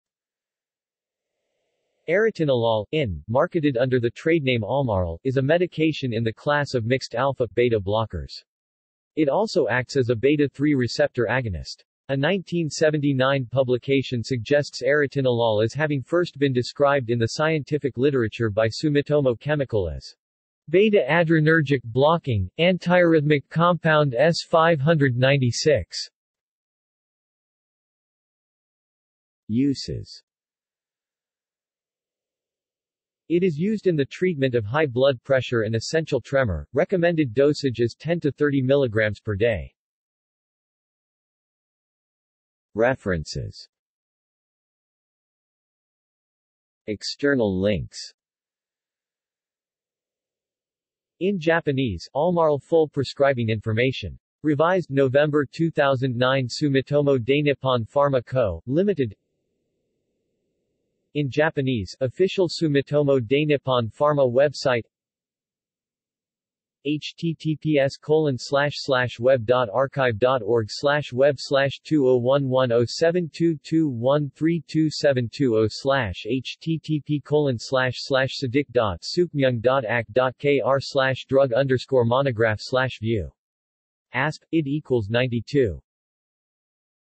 Arotinolol | Wikipedia audio article
This is an audio version of the Wikipedia Article:
Arotinolol

SUMMARY
Arotinolol (INN, marketed under the tradename Almarl) is a medication in the class of mixed alpha/beta blockers. It also acts as a β3 receptor agonist. A 1979 publication suggests arotinolol as having first been described in the scientific literature by Sumitomo Chemical as "β-adrenergic blocking, antiarrhythmic compound S-596".